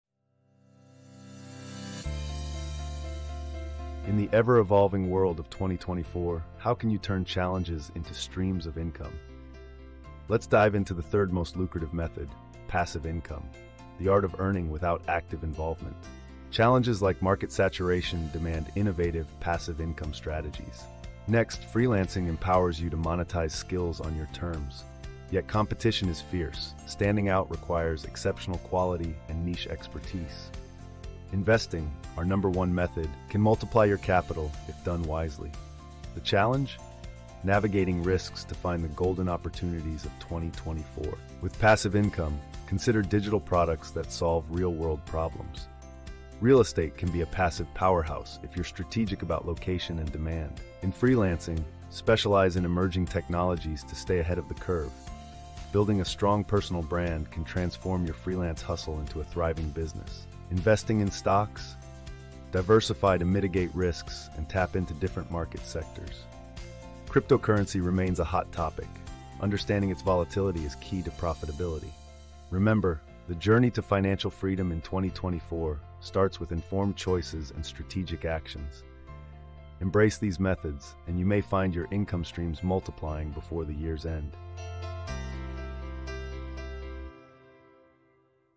Maximize Your Earnings: Innovative Money 💵 Making Strategies for 2024 🚀
Unlock the secrets to financial success in 2024 with our latest video: “Maximize Your Earnings: Innovative Money Making Strategies for 2024” 🚀💰 Dive into the world of Passive Income, Freelancing, and Investing as we guide you through lucrative opportunities and expert insights. 🌐💼 Whether it’s digital products, real estate, or staying ahead in freelancing, we’ve got you covered. Explore the realms of diversified stock investments and navigate the cryptocurrency landscape with confidence. 🏡📈 Stay informed, make strategic choices, and pave your way to financial freedom. Watch now and witness your income streams flourish by year’s end! FinancialFreedom MoneyMakingStrategies PassiveIncome Freelancing Investing2024 WealthBuilding InnovativeIncome Money 💵🔗